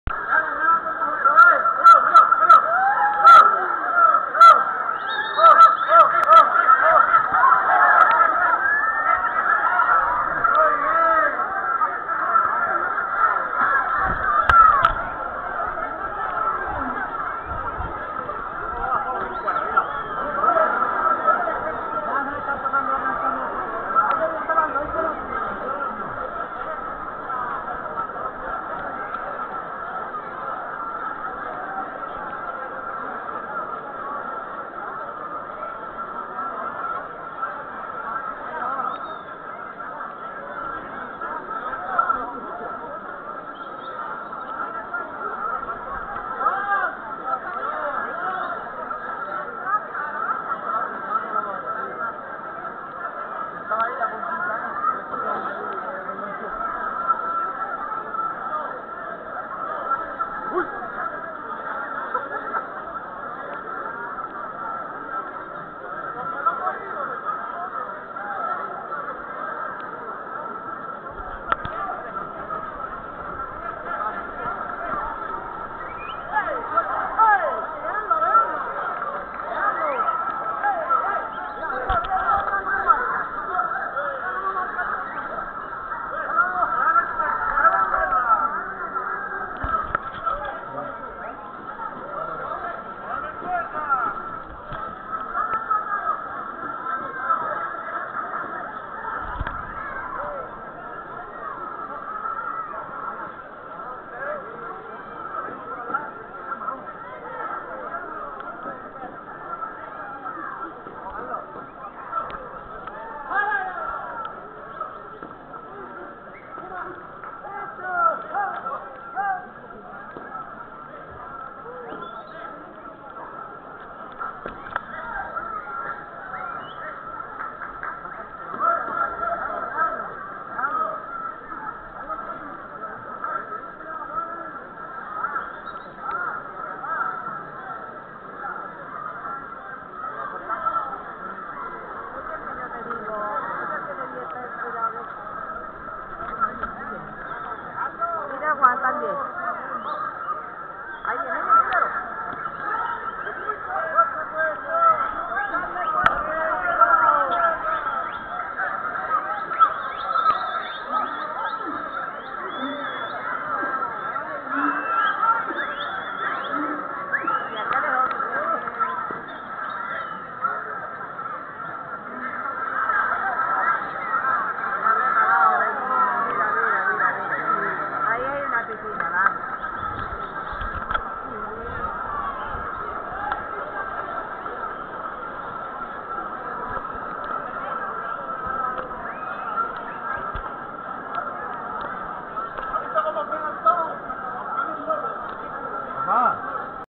Toro Enmaromado 25 Aniversario en Grazalema 2017.
Toro bajo la lluvia en el 25 Aniversario de la Peña Lunes del Toro de Cuerda, el día 29 de abril de 2017.